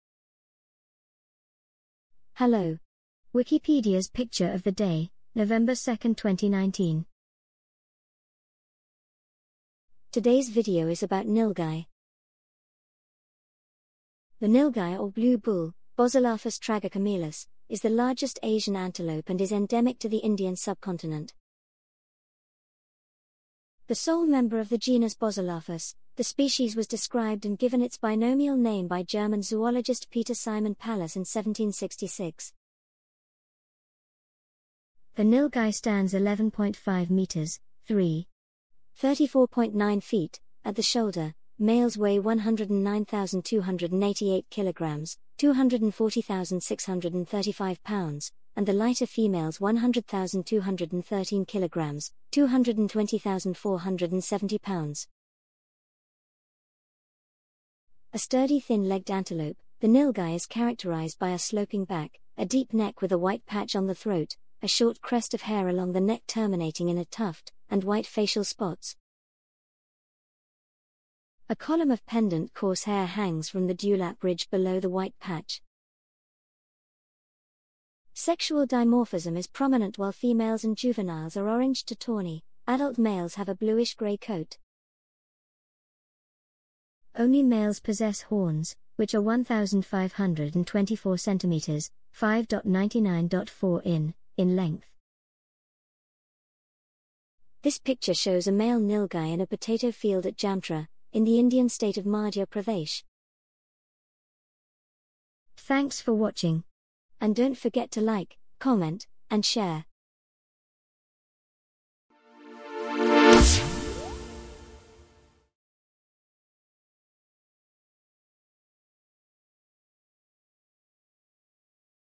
Wikipedia Picture of the Day: 2019-11-02 - Nilgai (Narrated by Emma)
Wikipedia Picture of the Day - November 2nd, 2019 - Nilgai (Narrated by Emma)

The nilgai or blue bull (Boselaphus tragocamelus) is the largest Asian antelope and is endemic to the Indian subcontinent. The sole member of the genus Boselaphus, the species was described and given its binomial name by German zoologist Peter Simon Pallas in 1766. The nilgai stands 1–1.5 metres (3.3–4.9 ft) at the shoulder; males weigh 109–288 kilograms (240–635 lb), and the lighter females 100–213 kilograms (220–470 lb). A sturdy thin-legged antelope, the nilgai is characterised by a sloping back, a deep neck with a white patch on the throat, a short crest of hair along the neck terminating in a tuft, and white facial spots. A column of pendant coarse hair hangs from the dewlap ridge below the white patch. Sexual dimorphism is prominent – while females and juveniles are orange to tawny, adult males have a bluish-grey coat. Only males possess horns, which are 15–24 centimetres (5.9–9.4 in) in length.
This picture shows a male nilgai in a potato field at Jamtra, in the Indian state of Madhya Pradesh.

Photograph credit: Charles J. Sharp